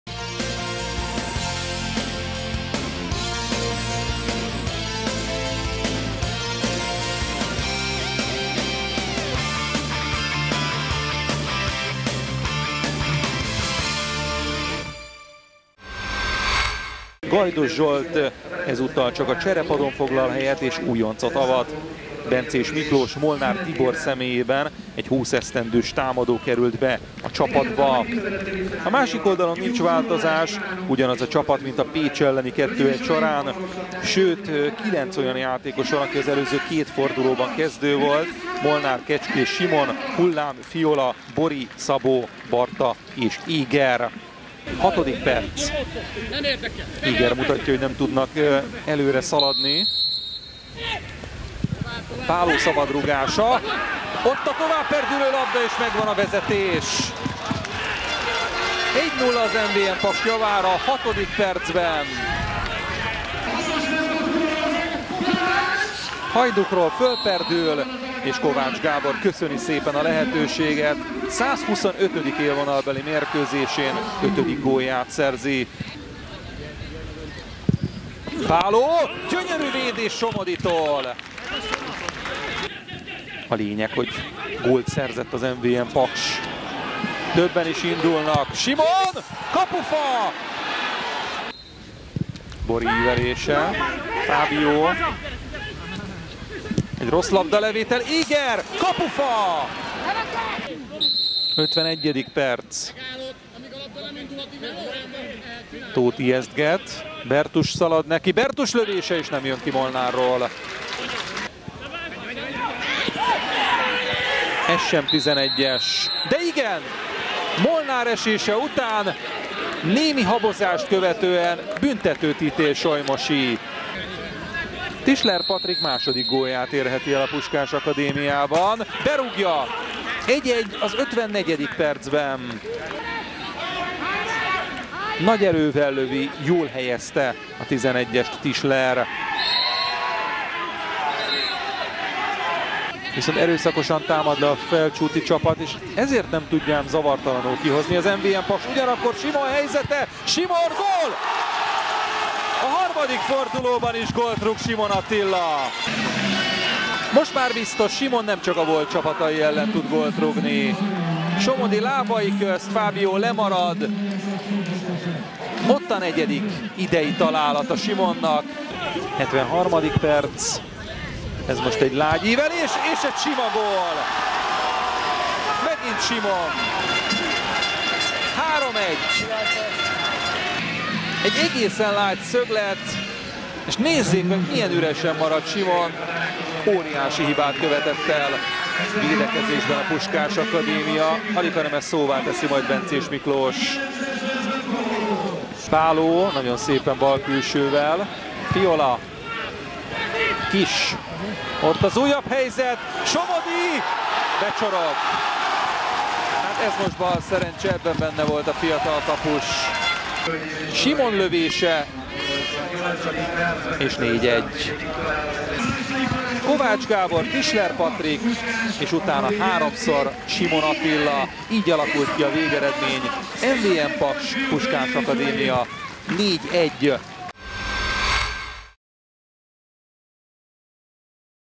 Tovább dübörög a Paks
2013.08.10 OTP BANK LIGA 3. forduló
MVM Paks-Piskás Akadémia 4-1
összefoglaló